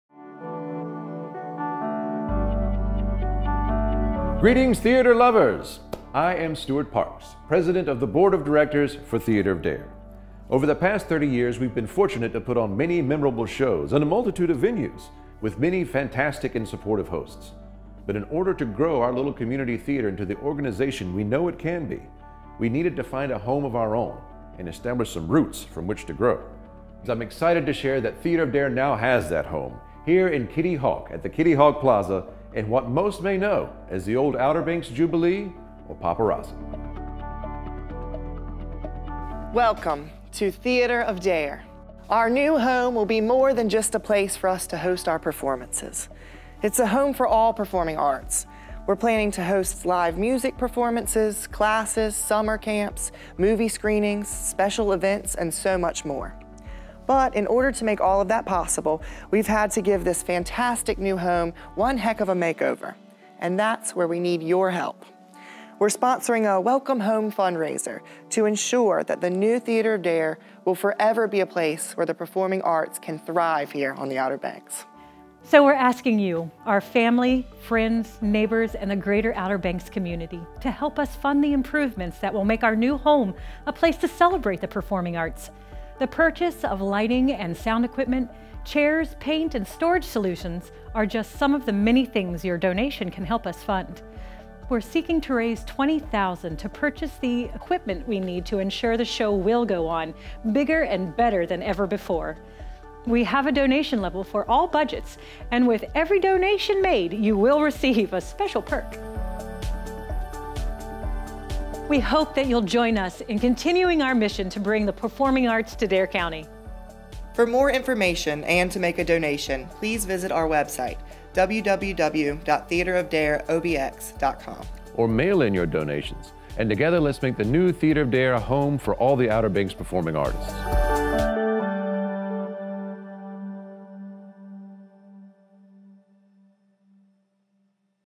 Welcome Home Fundraiser - Theatre of Dare
Theatre of Dare (TOD), community theater for the Outer Banks, will introduce its new home at the former Paparazzi OBX in Kitty Hawk with the opening of Arsenic and Old Lace on Friday, February 18, at 7:30 p.m.

In addition to quality theatrical productions for which the group has become known, the new TOD home is intended to be a place where all performing arts can be shared and celebrated. Plans include musical acts, classes in performing arts, summer camps, and much more. While casts and crews have been busy producing the first three shows of this 31st season, the board and other volunteers have been diligently working to successfully move into and remodel the new location.

In order to support the expenses involved in preparing the theater for use, TOD is launching a fundraising campaign to help defray the costs of technical equipment, particularly a much-needed theatrical lighting system, sound system upgrades, as well as chairs for the audience and building maintenance requirements.

Our goal is to raise $20,000, all of which will go to cover the cost of items we need to pull off successful productions in our new home.  Plus, there will be incentive perks as a ‘thank you’ at each level of giving.